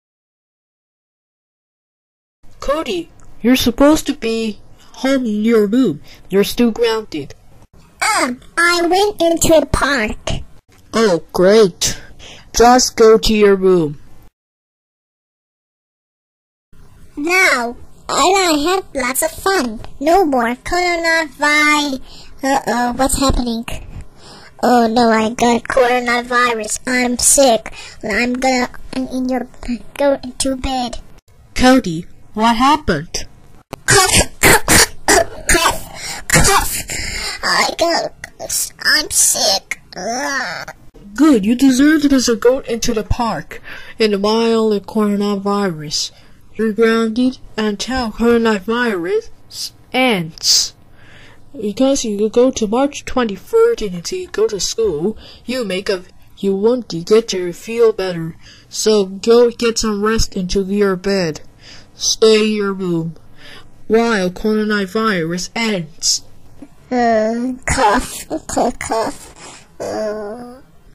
Cody Gets Sick/Coronavirus
Cody Went To the Park While Staying Home/Coronavirus, And Now Cody Is Sick For Going To The Park